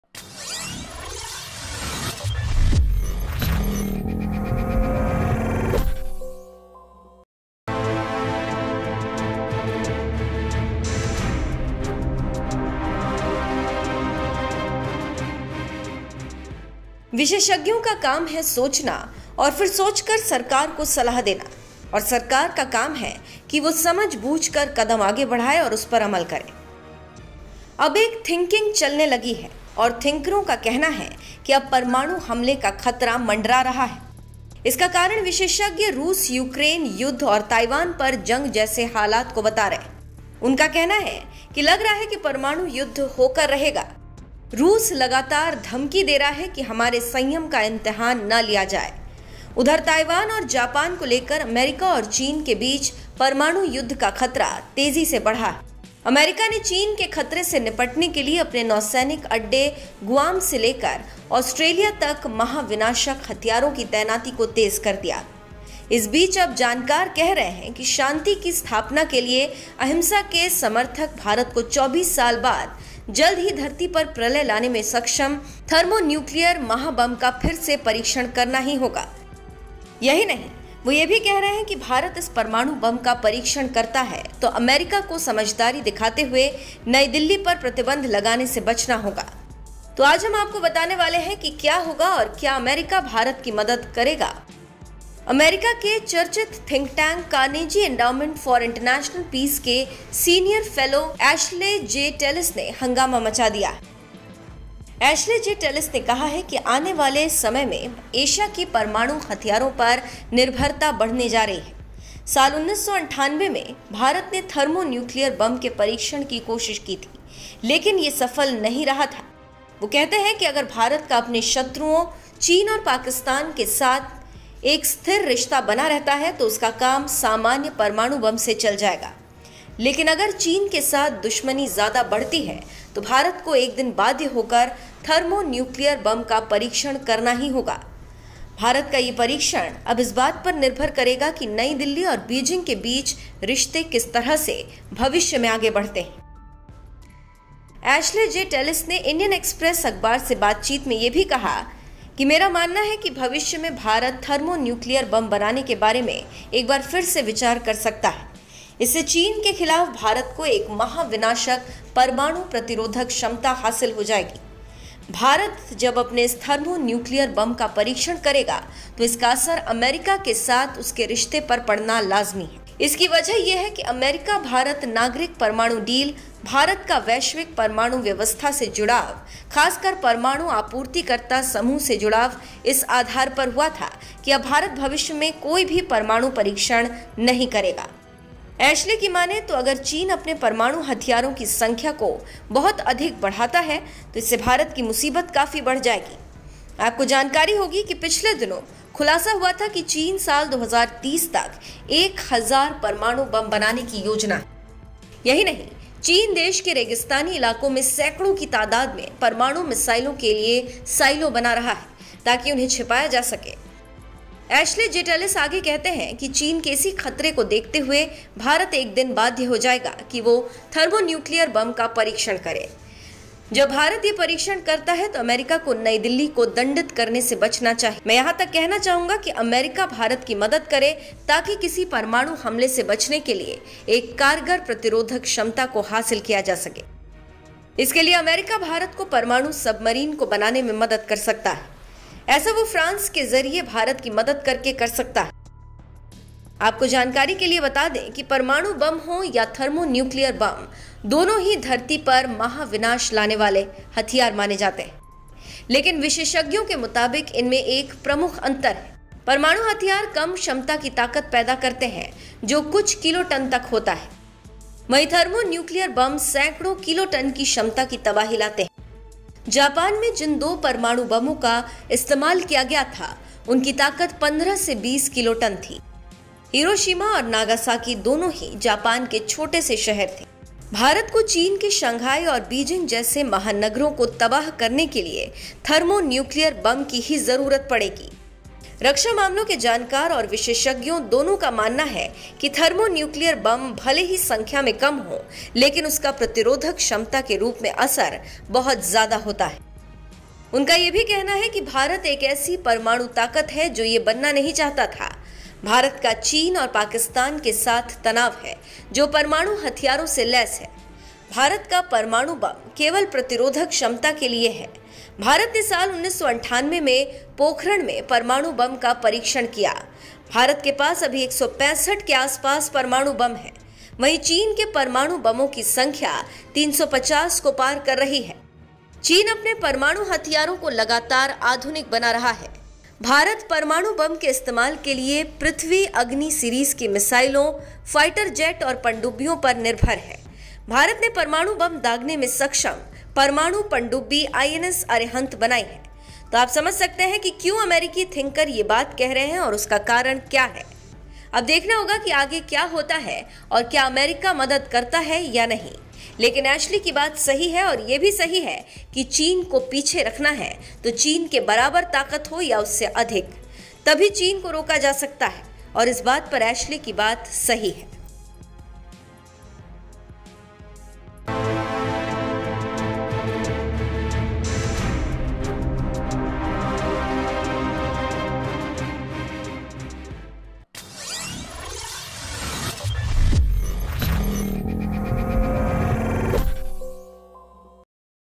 भारत करेगा महाविनाशक थर्मोन्‍यूक्लियर बम का परीक्षण. अमेरिका देगा साथ ?
रूस-यूक्रेन युद्ध और ताइवान पर जंग जैसे हालात के बीच दुनियाभर में परमाणु युद्ध का खतरा बढ़ता जा रहा है… रूस लगातार धमकी दे रहा है कि हमारे संयम का इम्तिहान न लिया जाए… उधर, ताइवान और जापान को लेकर को लेकर अमेरिका और चीन के बीच परमाणु युद्ध का खतरा तेजी से बढ़ा है…

Amin
kk